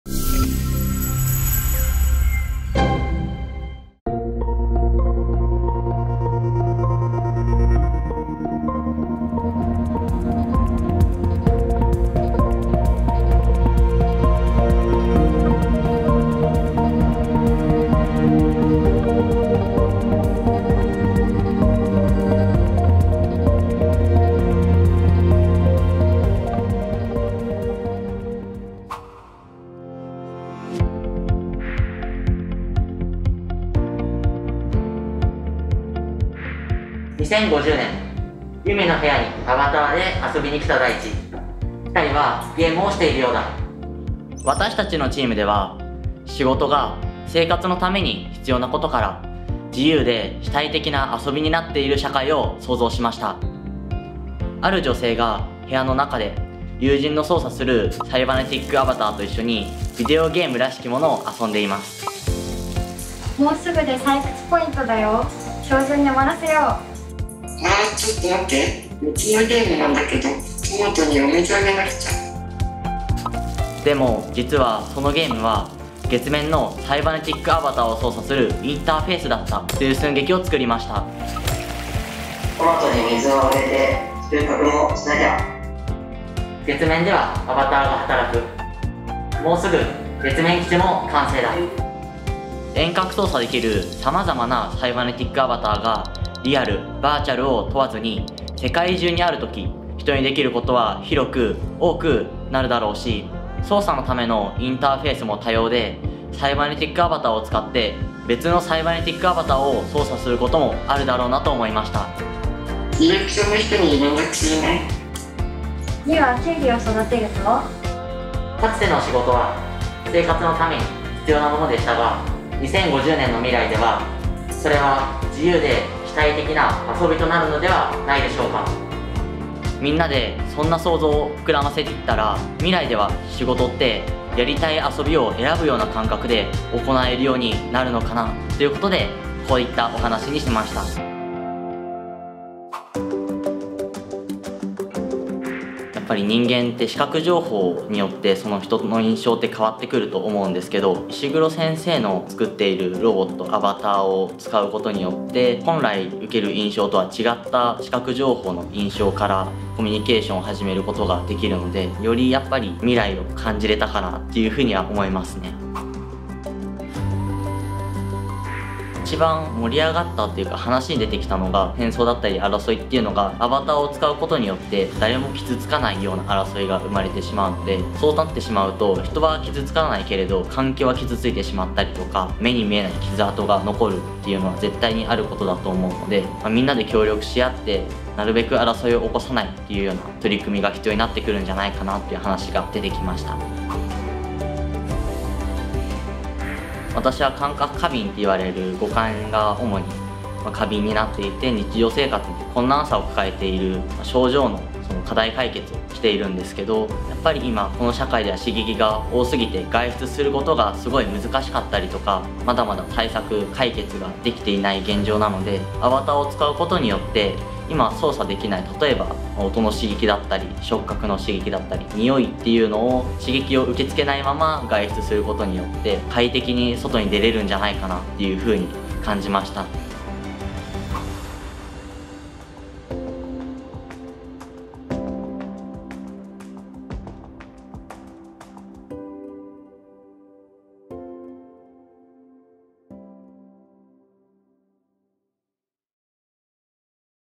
#02 仕事が「必要なこと」から「遊び」になる社会 OUR FUTURE LIFE WITH CYBERNETIC AVATARS
0:00 オープニング
0:30 寸劇発表「仕事が「必要なこと」から「遊び」になる社会」
2:23 インタビュー

ムーンショット型研究開発事業の目標1は、2050年までに人が身体、脳、空間、時間の制約から解放された社会を実現することを目標にしています。
そのために、サイボーグやアバターとして知られる一連の技術を高度に活用し、人の身体的能力、認知能力及び知覚能力を拡張するサイバネティック・アバター（CA）技術の研究開発を推進しています。
今回、未来を担う若者たちを対象に、デモ体験や1日のワークショップを開催し、CA技術によって実現される未来社会がどのような社会になるのかを議論し、若者たちの考える未来社会を寸劇形式で発表し合いました。